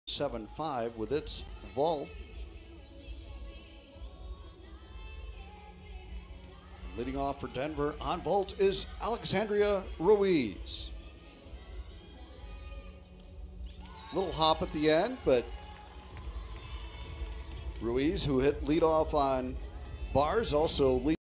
Alexandria Ruiz - Vault (9.700) - 2021 DU Gymnastics at Iowa State
University of Denver gymnast Alexandria Ruiz competes on vault against Iowa State in Hilton Coliseum (Ames, Iowa), on January 15, 2021. Score: 9.700